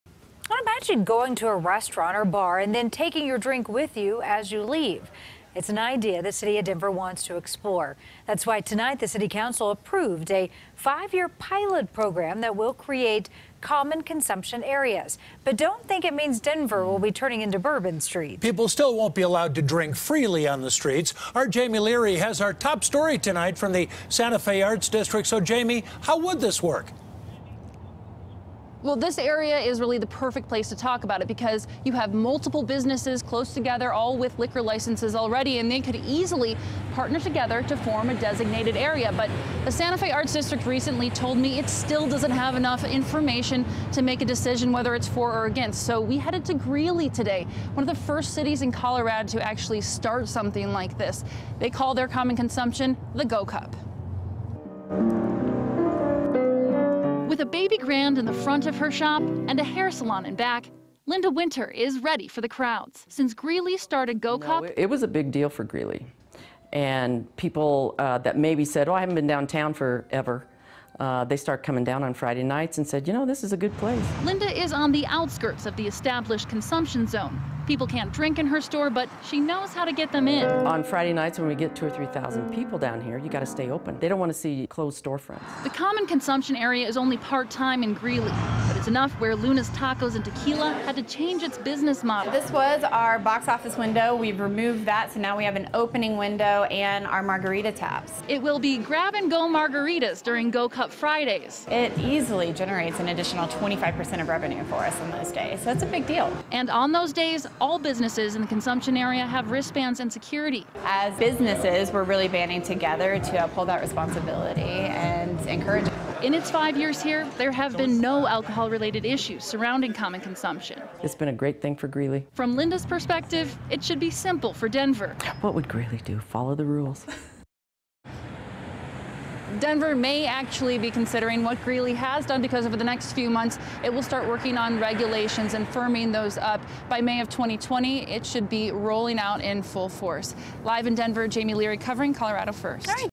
Denver Passes Bill For Common Consumption Areas Similar To Greeley's
The city of Greeley was the first municipality in the state to pass a similar proposal.